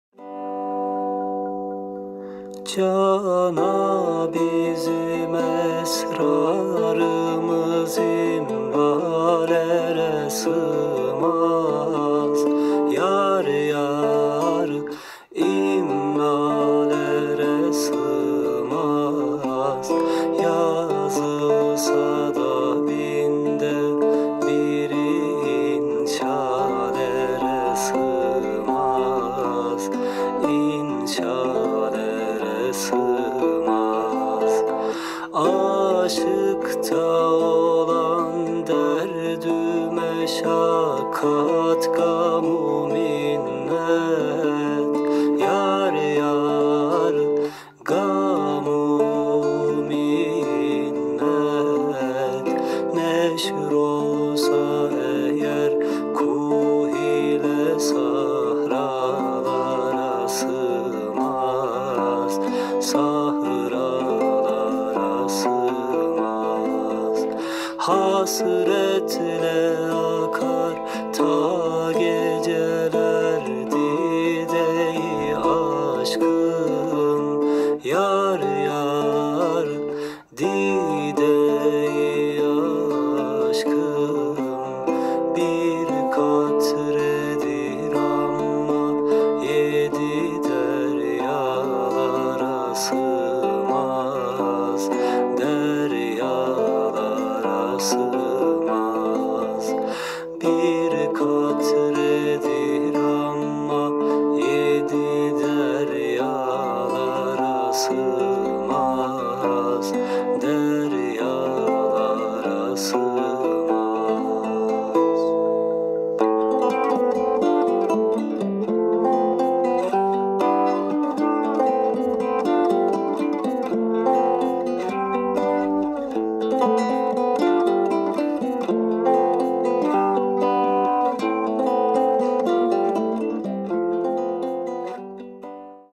Meşk / Besmele Duvar Çizimi
Yeryüzüne sığmayan gönlüme kanatlar çizdim.
'Saldım gönlümü gökyüzüne arşı dolansın.. '
Çizimime eşlik eden sesin sahibi : Taner Baba